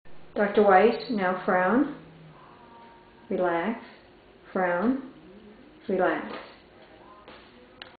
Dr. Richard Weiss Gets Botox Cosmetic - Before
This is a video of Richard Weiss, MD of Newport Beach, CA attempting to frown before a glabellar (between the eyebrows) Botox Cosmetic treatment.